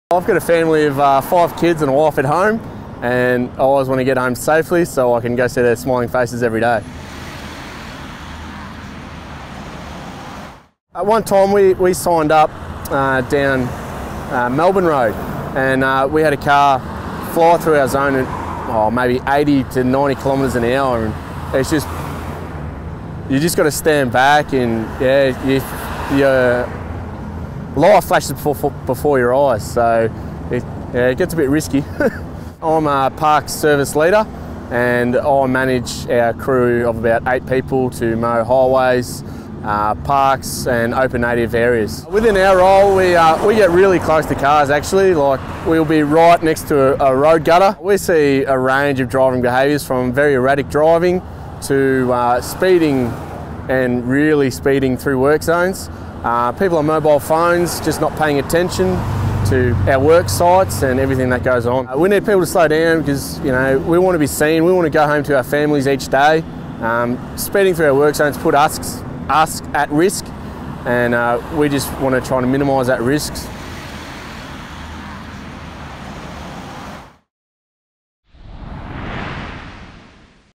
Real Workers, Real Speed Limits - Louis
Louis wants to go home to his partner and kids. Drivers need to slow down while going through work zones so that our staff can be safe and get home to their families.